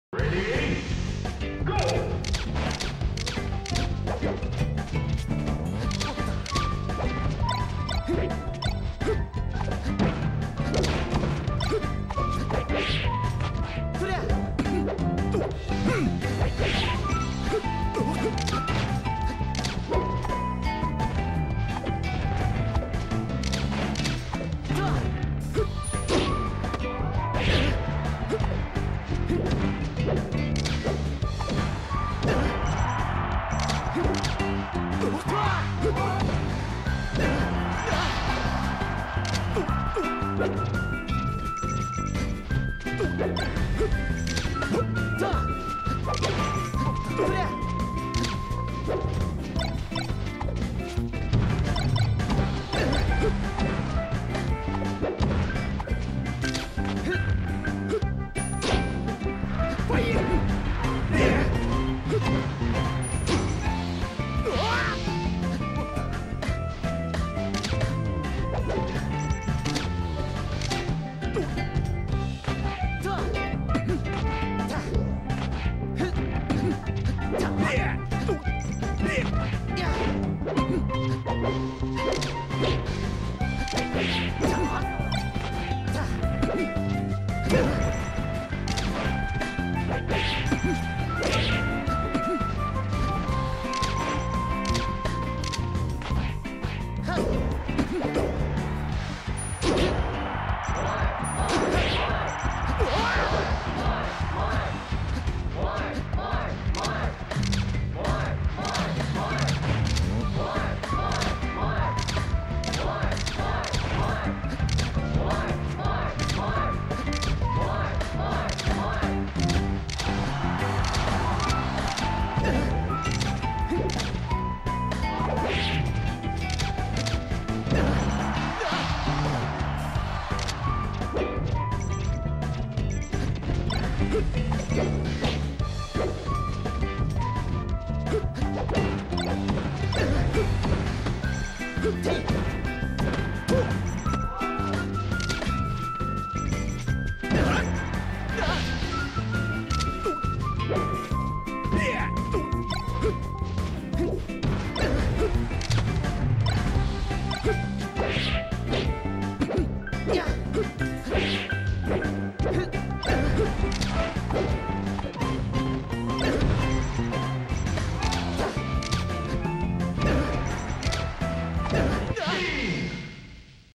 DaShizWiz vs Dark Falchion
Gigabits 3/17 Singles
Grand Finals Set 2
DaShizWiz (Falco) vs DarkFalchion (Marth)